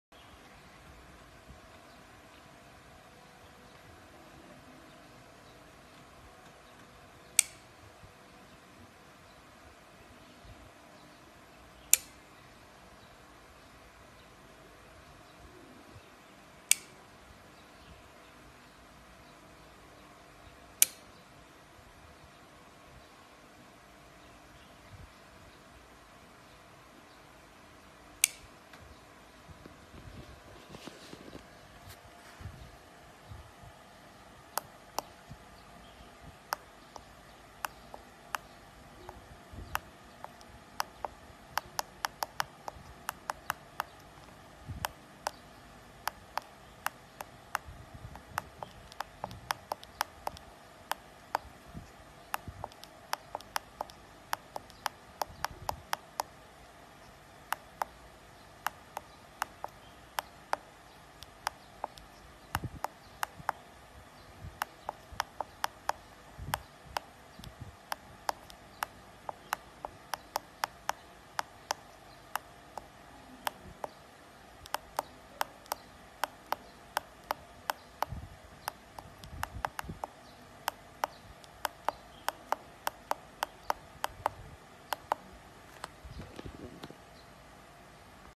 Over Voltage and Under Voltage Protector with Over loads
The Over Voltage and Under Voltage Protector with Over loads. It can set the Over Voltage from 240 to 300VAC, Under Voltage from 140 to 200VAC, Relay on time from 3 to 60 seconds and Over loads resets time from 30 to 180 seconds.